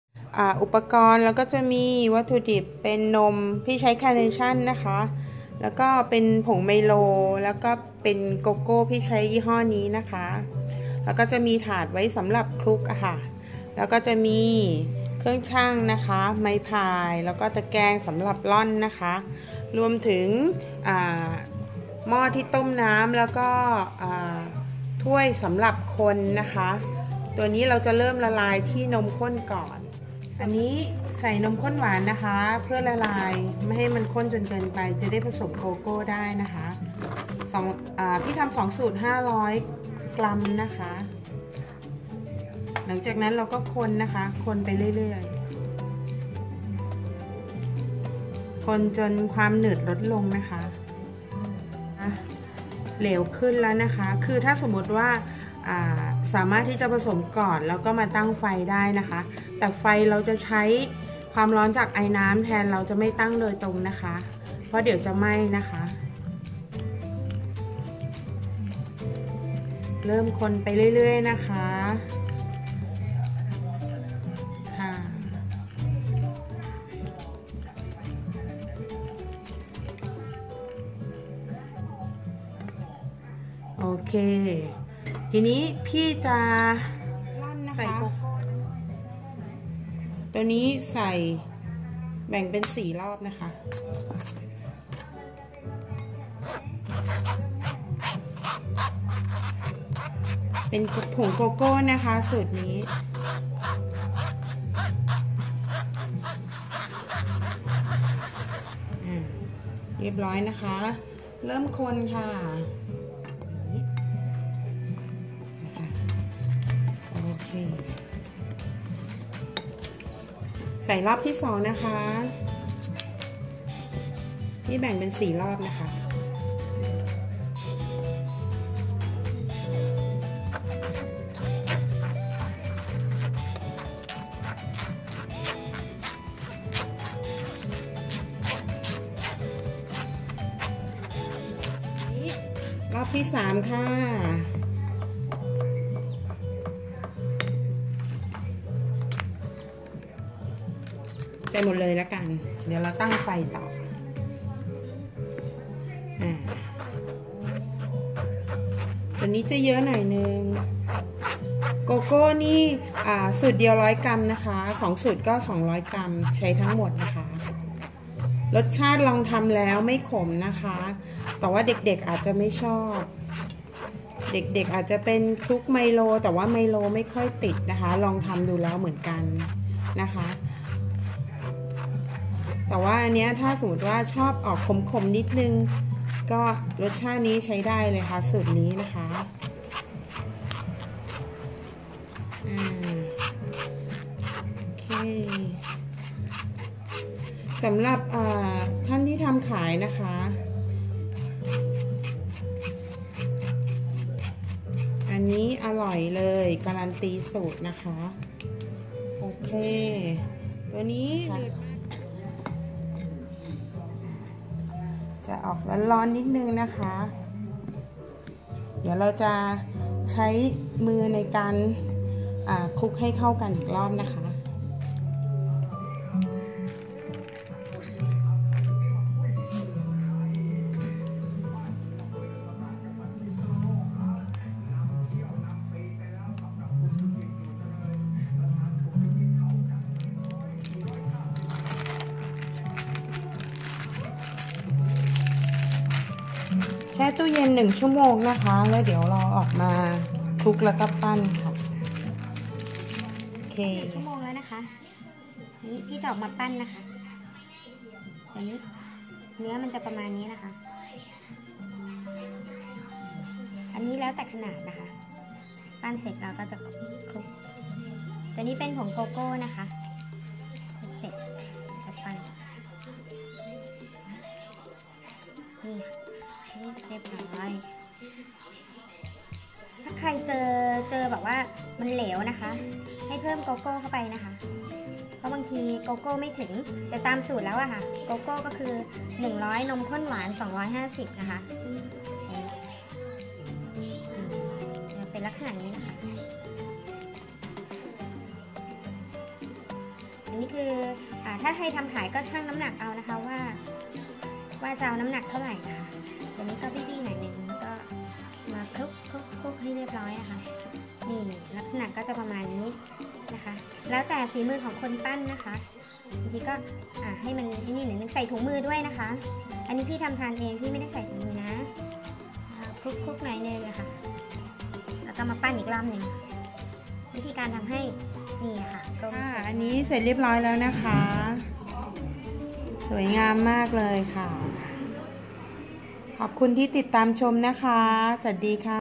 สอนทำโกโก้หนึบ ทำขายกำไรดีมาก เด็กหอก็ทำได้ ส่วนผสมแค่สองอย่าง ออเดอร์ทำไม่ทัน อร่อยปังมาก
ครัวข้าวสวย by mae Muay สอนทำโกโก้หนึบ
ทำขาย กำไรงาม ส่วนผสมหาง่าย วิธีทำง่ายมาก
ึคำแนะนำ ลองเปลี่ยน วัตถุดิบ จากโกโก้เป็นไมโลแทน ปรากฏว่า ส่วนผสมเหลวนะคะ ส่วนตัวไม่แนะนำ ถ้าจะนำมาคลุกให้ผสมผงโกโก้ด้วยจ้าเพราะคลุกแค่ ผงไมโลหรือโอวัลตินไม่ค่อยติด
วิธีทำ 1 สูตร
1. ตวงนมข้นหวาน 250  กรัม
2. ตวง ผงโกโก้ 100 กรัม
3.นำหม้อตั้งไฟ เอาถ้วยใส่ นมข้นหวานตั้งลงไปในหม้อ ทำการคน จนไม่หนืด หรือจะใช้ไมโครเวฟในการละลายได้ จ้า
4.นำผงโกโก้ แบ่งร่อนใส่ ถ้วยนำข้นหวานที่ยกลงมาแล้ว แบ่งร่อนเป็น 3-4 รอบ ค่อยๆคน เมื่อใส่ผงโกโก้หมดแล้ว
จะออกหนืดๆ ให้ เอามาวางบนพลาสติกสำหรับอาหารเพื่อนำเข้าตู้เย็น ช่องธรรดา
5.แช่ไว้ 1 ชม.
6.นำออกมาคลุกกับผงโกโก้ก่อน จากนั้น ค่อยปั้นเป็นลูกกลมๆ แล้วนำไปคลุกผงโกโก้อีกที
7.นำใส่กล่องแช่ตู้เย็น
หมายเหตุ การวางไว้ข้างนอกตู้เย็น จะทำให้ผิวไม่สวยและ ก้อนไม่แข็ง ออกเหลวนิดนึง
การคลุก สา่มารถใช้ ผงไมโล โอวัลตินผสม กันแล้วคลุกได้ แต่ ถ้าใช้ ผงไมโลหรือโอวัลตินอย่างเดียว ไม่ค่อยติด จ้า
น้ำหนักต่อลูกประมาณ 25-30 กรัม พี่ใส่กล่อง 6 ลูก ขาย 30 บาท
สอดไส้ได้นะคะ เป็นพวกถั่ว ต่างๆ หรือนมข้นหวานก็ ทำเป็นไส้ได้ จ้า
ขมไหม ส่วนตัว คิดว่า ขมตัดกับหวานกำลังดี
ขอบคุณทุกๆท่านที่ติดตาม จ้า